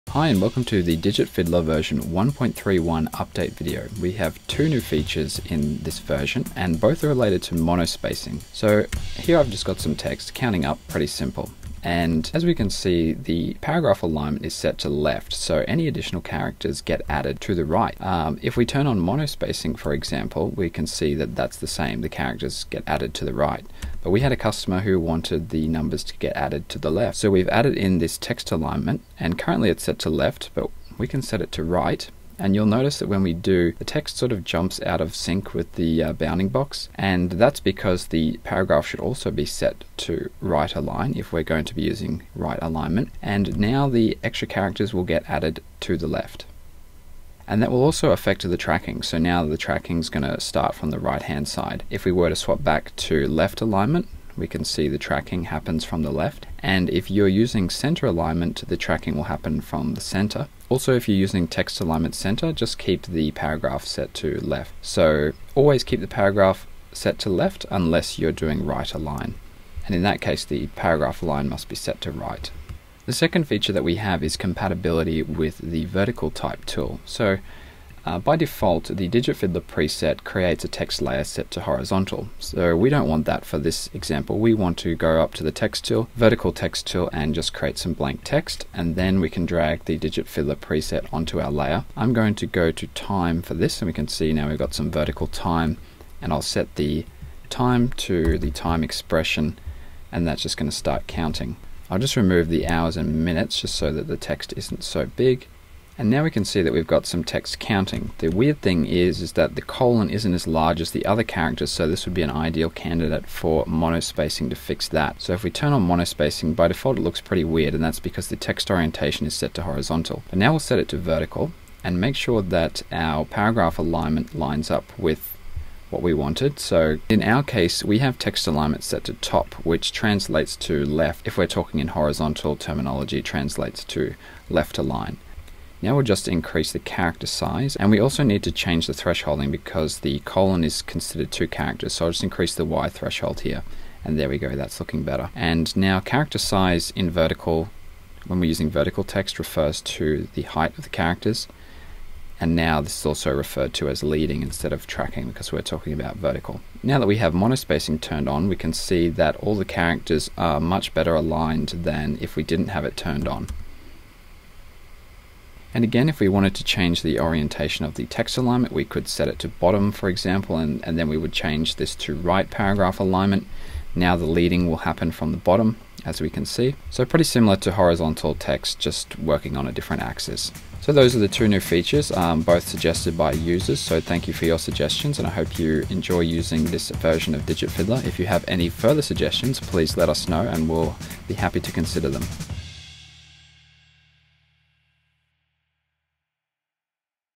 Digit Fiddler v1.3.1 New Features Tutorial - Available now on aescripts
This video covers the two new features included in the Digit Fiddler v1.3.1 update. Paragraph align and vertical text support for monospacing.

SOCIAL